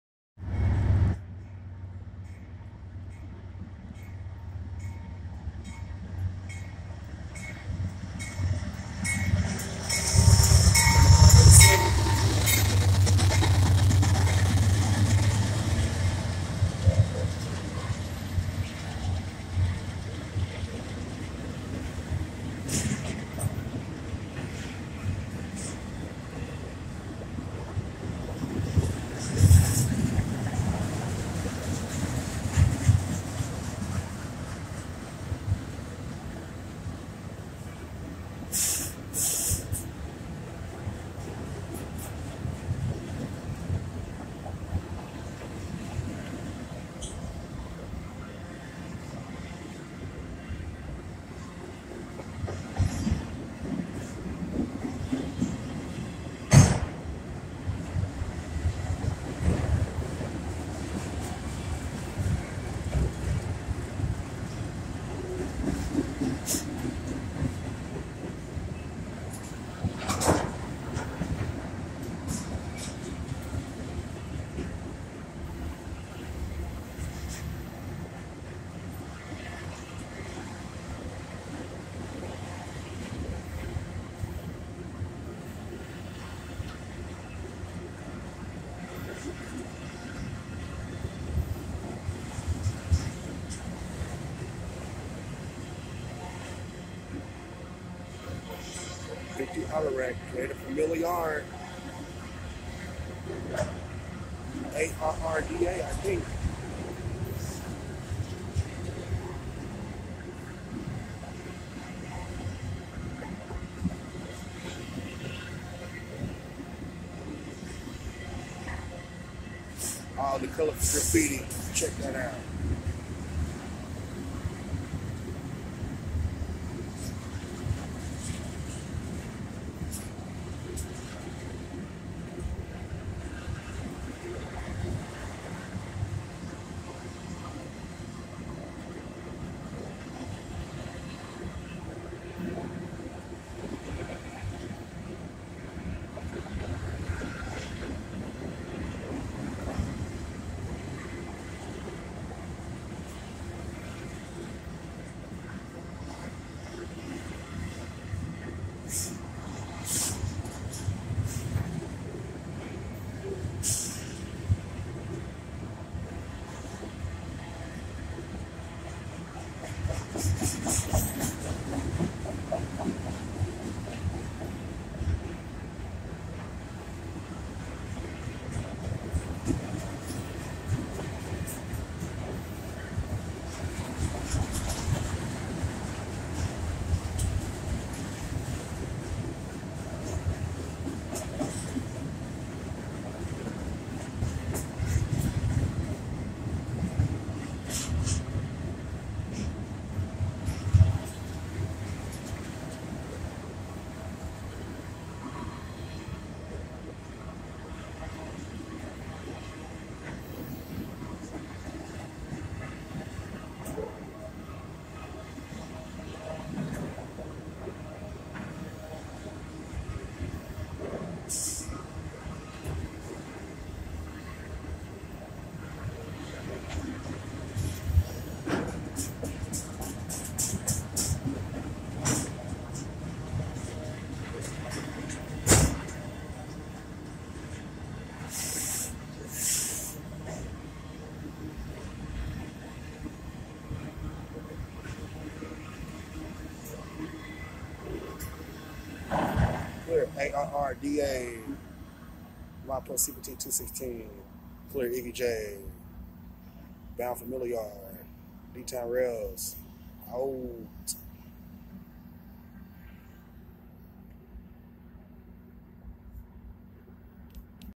UP 9587 Fresh Paint Solo
Beautiful  Day Of Railfanning  At EBJ Union Station  As  A Empty  AutoRack Train Passes On Track 2  with Art  Work on Every  single  Car

Loco's
UP  9587 AC44  With  Fresh Paint